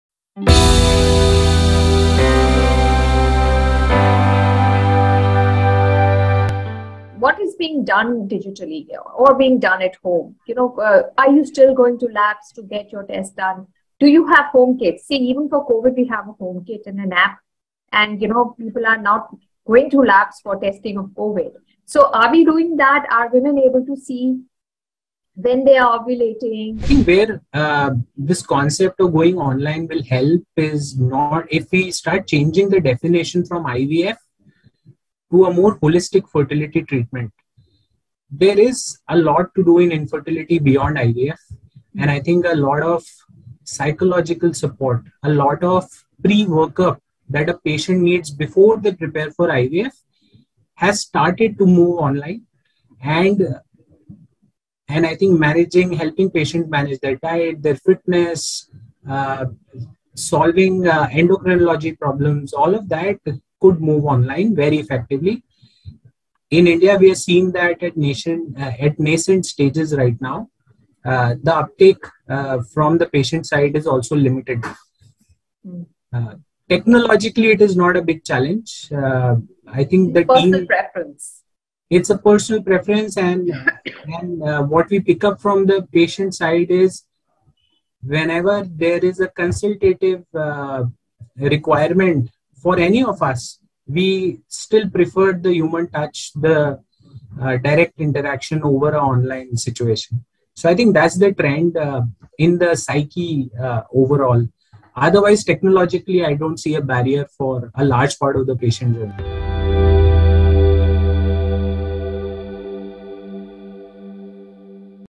Are we able to Digitize the IVF process? | Mr Abhishek Aggarwal | India IVF Summit
Here Mr Abhishek Aggrawal, Chief Business Officer, @birlafertilityivf-CK Birla Group talks about the IVF process going online in future with the technology getting advanced, where it is more accessible to the patients all over the country and the cost also gets reduced. This also attracts more patients towards IVF.

Watch in detail - India IVF Summit 2022, an initiative by IHW Council and @bharatserumsandvaccineslimited